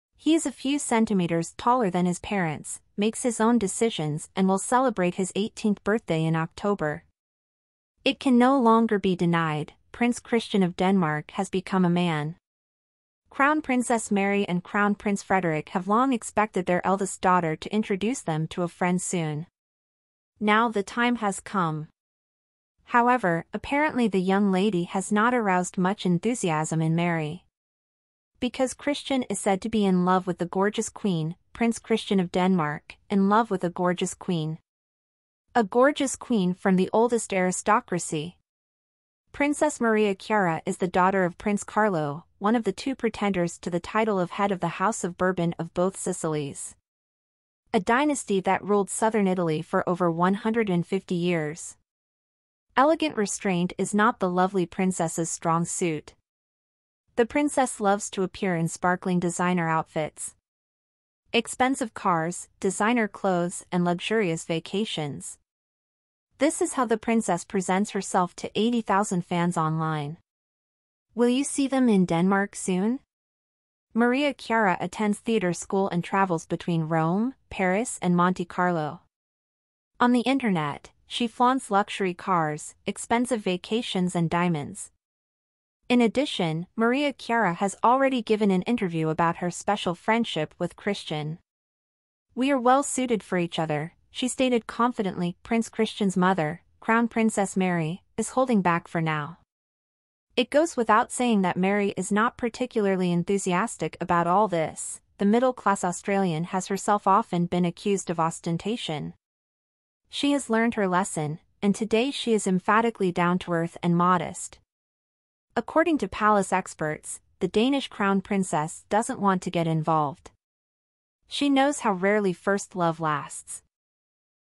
DENMARK'S PRINCESS MARY IS FURIOUS OVER NEW DAUGHTER-IN-LAW
He is a few centimeters taller than his parents, makes his own decisions and will celebrate his 18th birthday in October. It can no longer be denied: Prince Christian of Denmark has become a man. Crown Princess Mary and Crown Prince Frederik have long expected their eldest daughter to introduce them to a friend soon. Now the time has come.